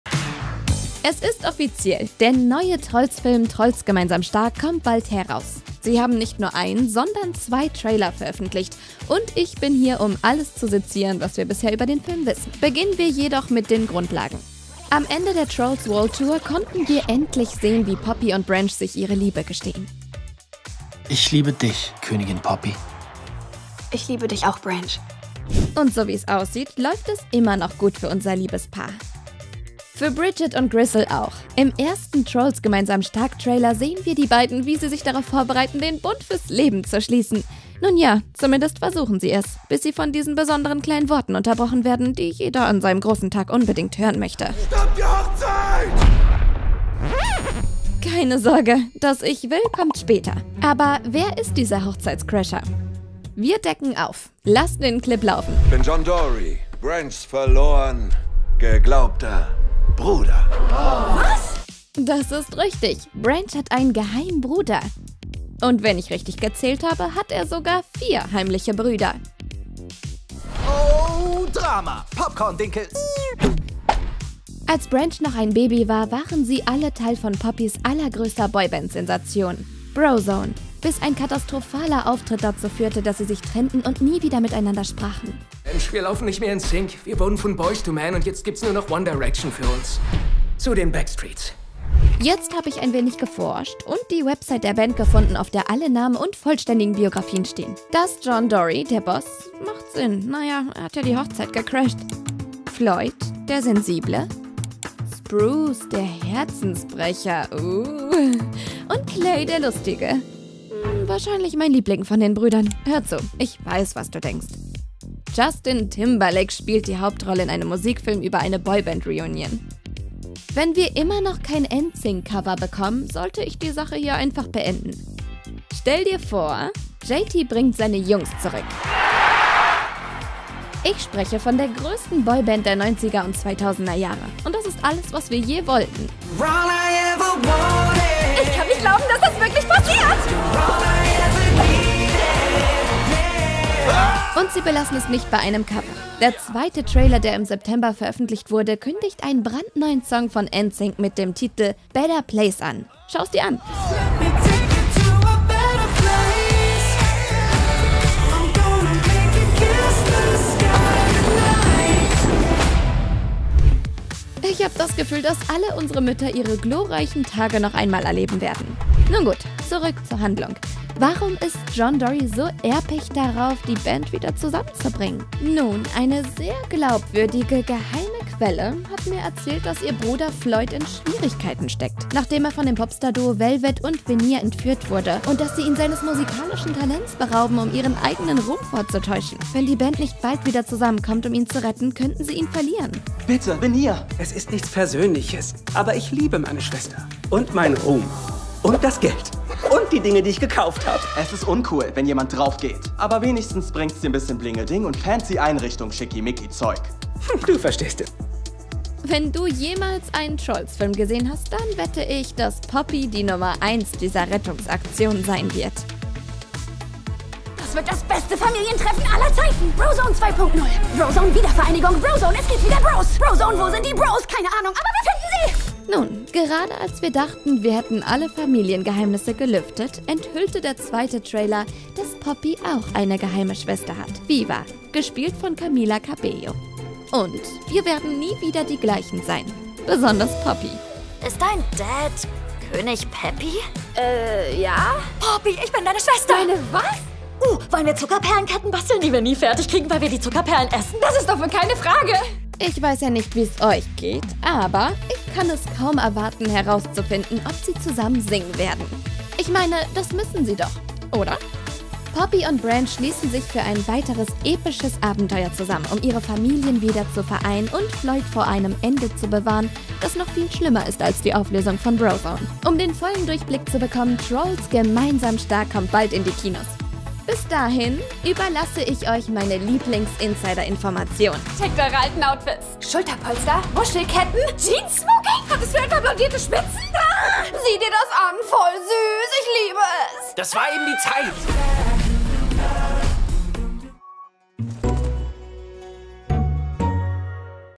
Alles, was wir über TROLLS–GEMEINSAM STARK wissen!
Trolls - Gemeinsam Stark kommt bald in die Kinos und wir verraten euch alles, was wir bisher über den Film wissen!